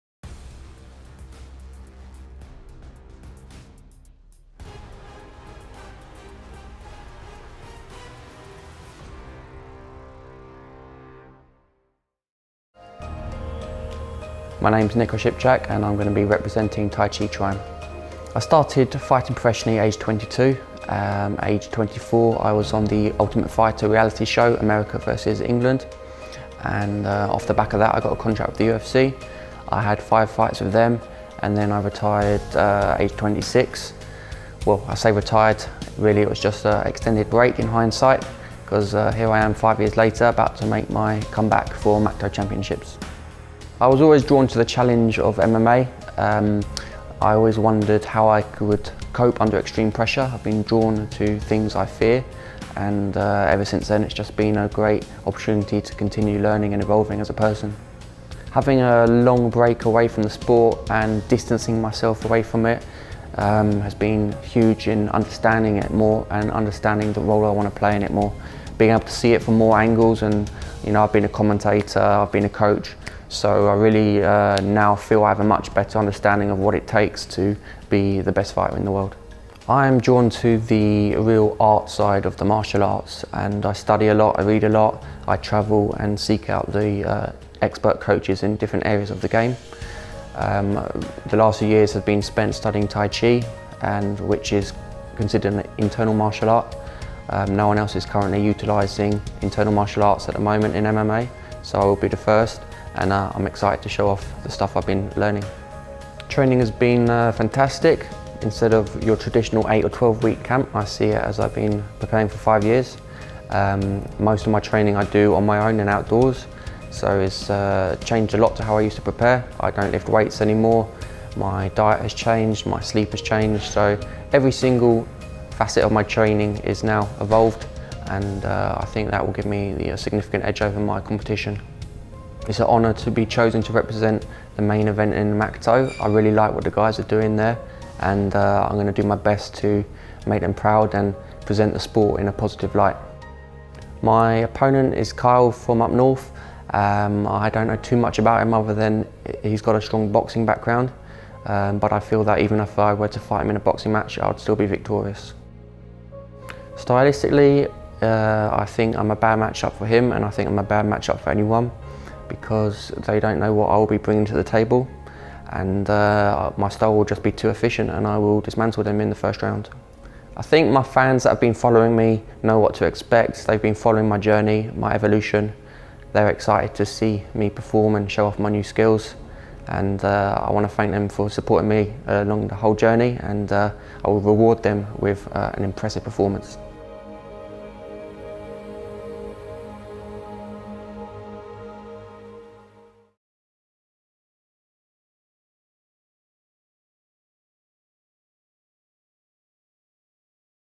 "Get to know" Nick Osipczak
This promo video was released by Macto Championships leading up to their inaugural event, which Nick headlined June 27th 2015.
He defeated his opponent Kyle Redfearn by submission, 2 minutes 4 seconds into the first round, and in doing so became the first person in History to represent the Internal Martial Arts in the sport of MMA.